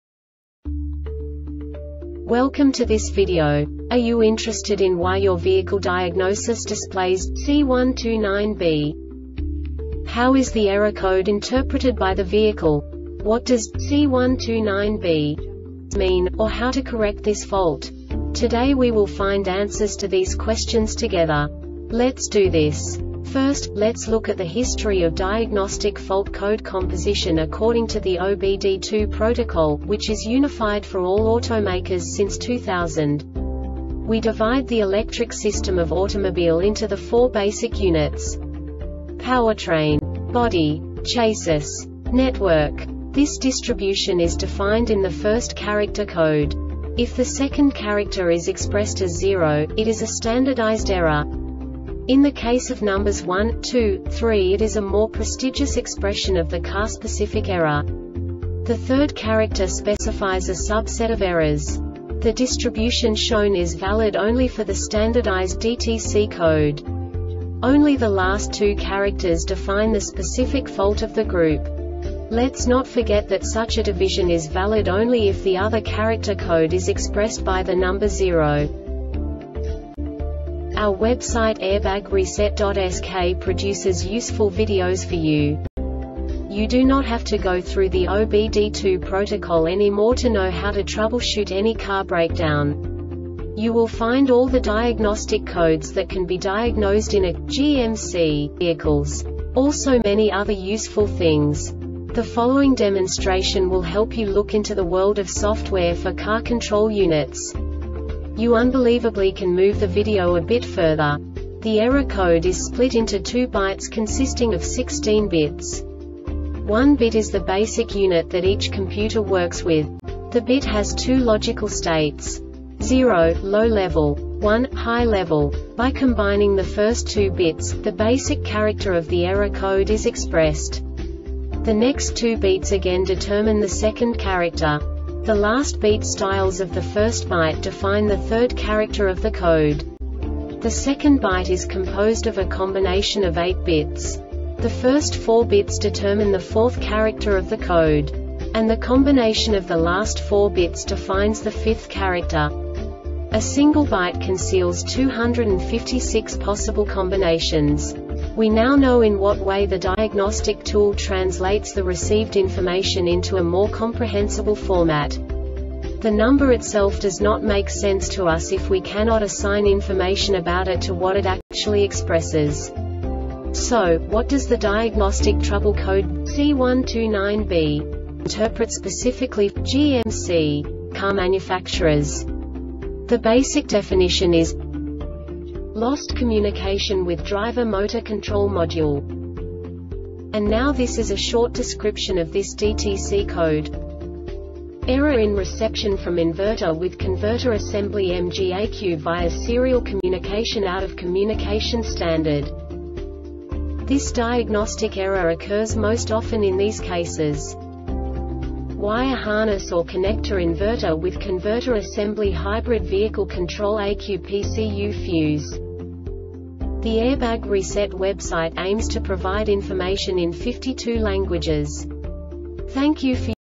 DTC GMC C129B Short Explanation
Contents:
0:21 Basic DTC analysis according to OBD2 protocol standard.
1:48 Insight into programming
2:58 A diagnostically understandable description of the Diagnostic Trouble Code
3:05 Basic error definition

Make:
GMC
Code:
C129B
Definition:
Lost Communication with Driver Motor Control Module
Description:
Error in reception from inverter with converter assembly (MG ECU) via serial communication (out of communication standard)
Cause:
 Wire harness or connector Inverter with converter assembly Hybrid vehicle control ECU PCU fuse